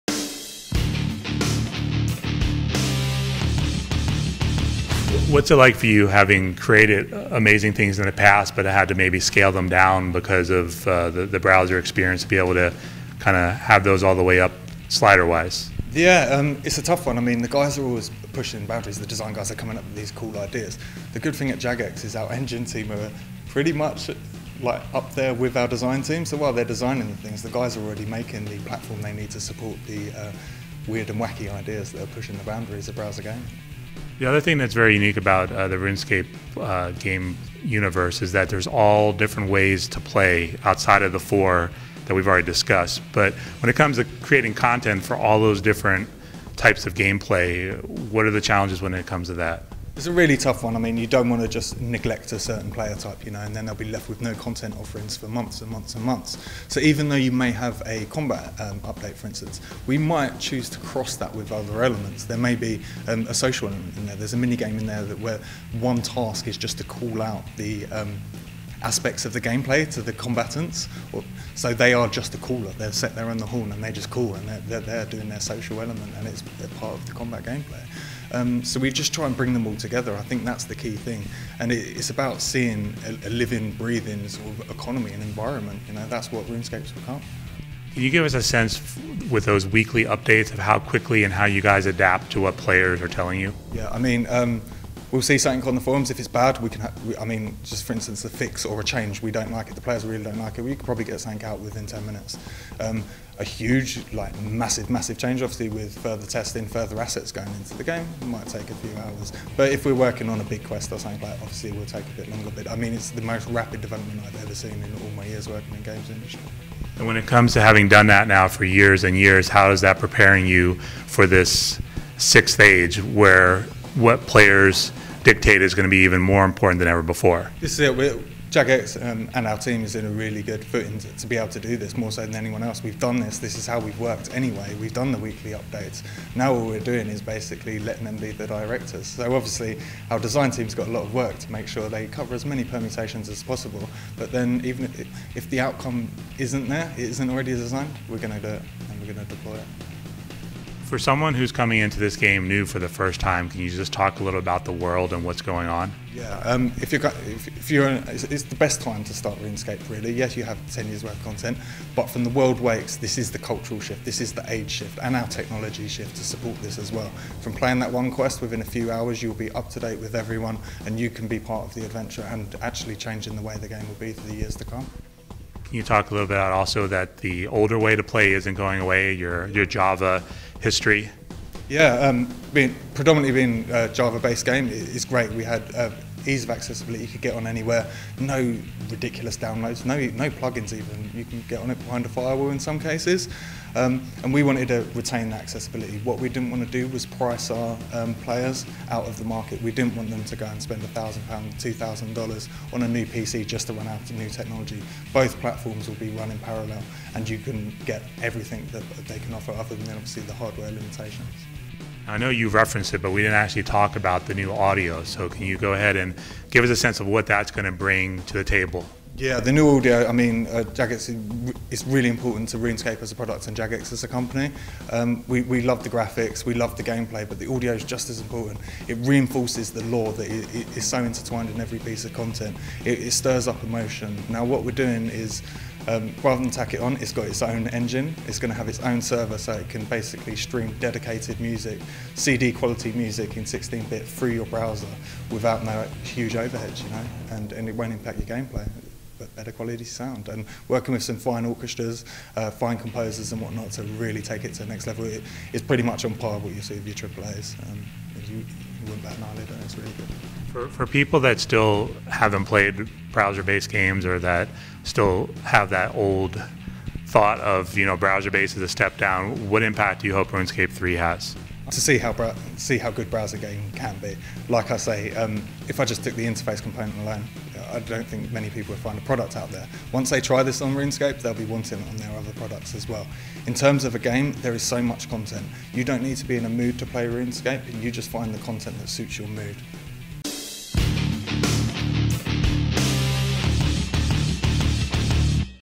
Runescape 3 Developer Dean Ollive Explores The New Fantasy World - Gamerhubtv
At Peckforton Castle in Cheshire, Jagex content development manager Dean Ollive previews the new Runescape 3 free to play game in this exclusive interview.